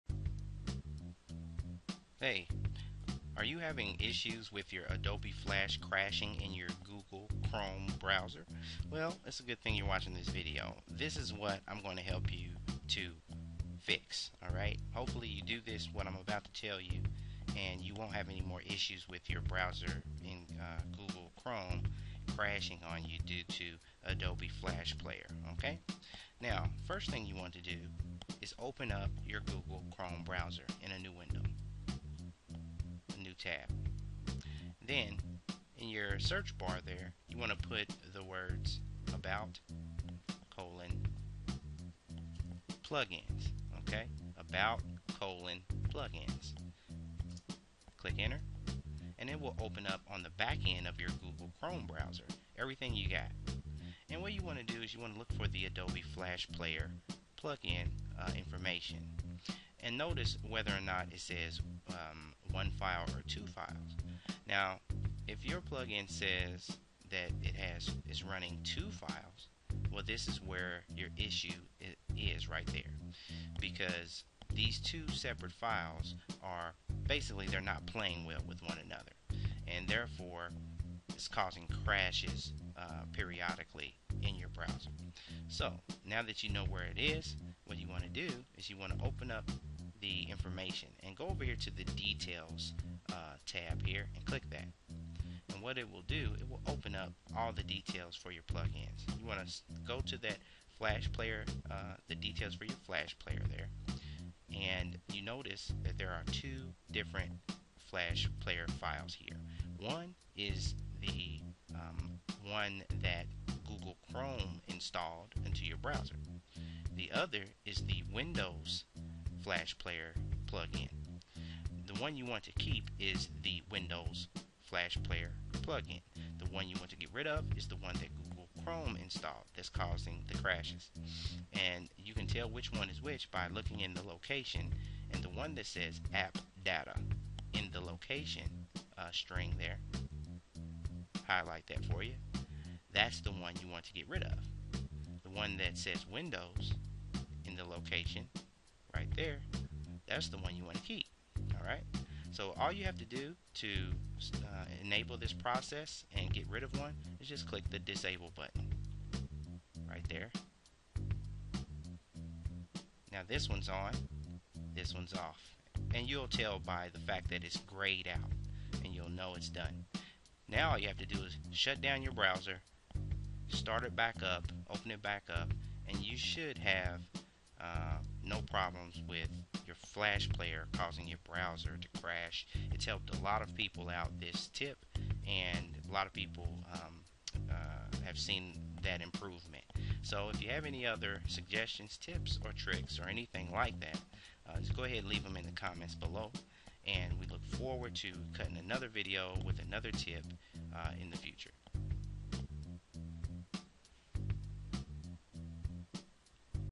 How to fix crashes from adobe flash player in google chrome browser
Hopefully this helps you out. If you would like to get info on another fix for your computer just put it in the comments and we'll see if we can find it for you ok.